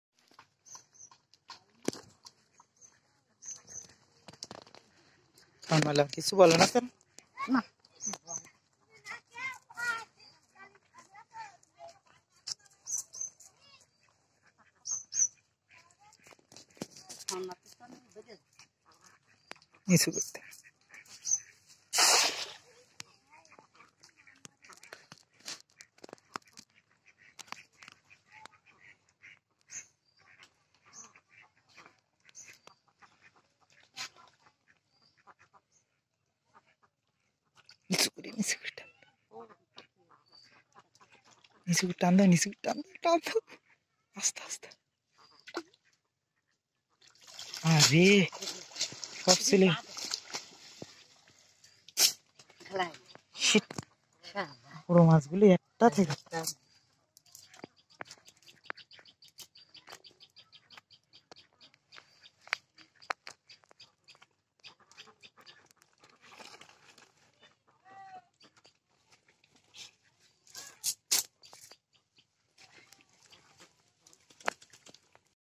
Net Fishing | See and learn how to catch shrimp fish from the pond | All Fish Bangla TV | part : 15
# net Fishing
# big pond
# village pond
# woman
# beautiful woman